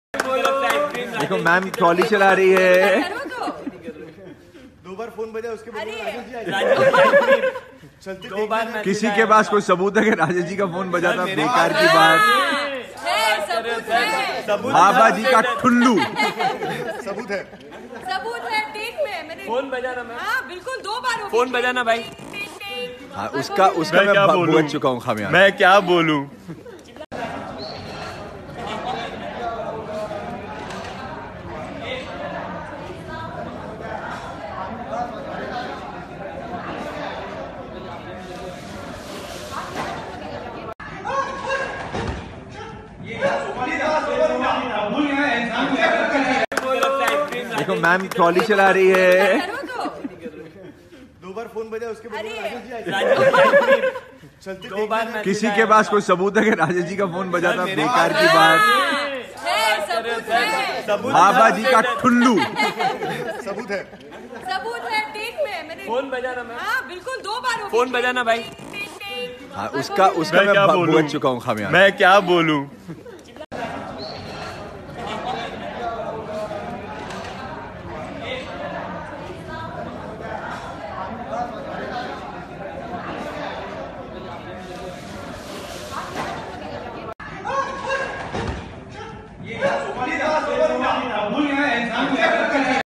Bepannah: Shooting में अचानक बजा mobile phone तो Zoya aka Jennifer ने दी सजा
Bepannah: Shooting में अचानक बजा mobile phone तो Zoya aka Jennifer ने दी ऐसी सजा